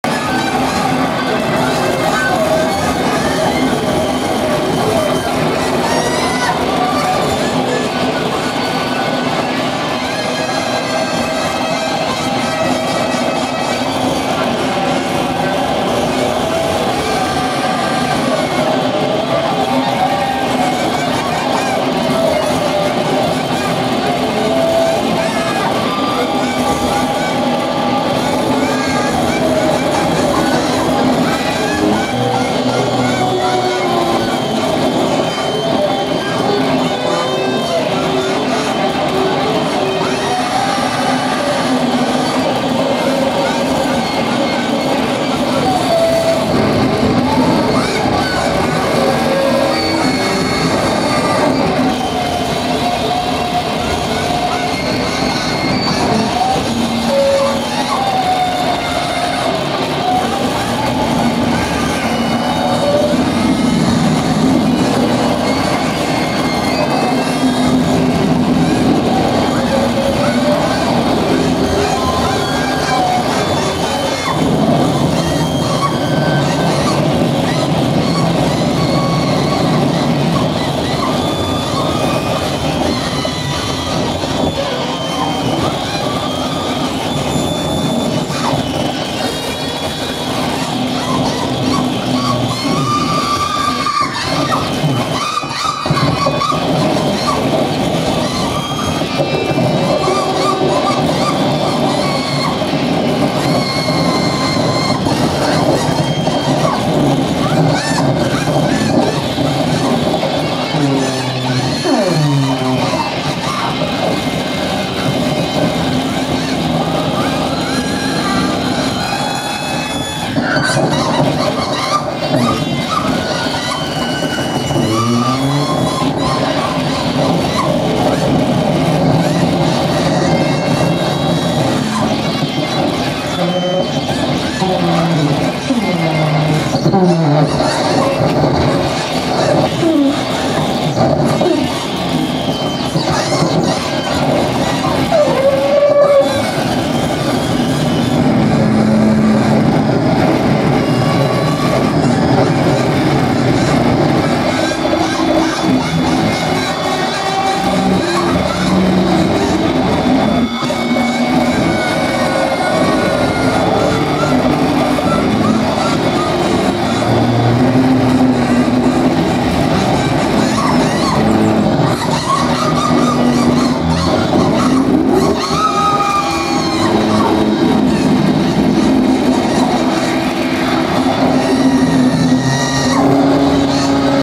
Hijokaidan
Jojo, Junko, and T. Mikawa, at The Handbag Factory in Los Angeles, CA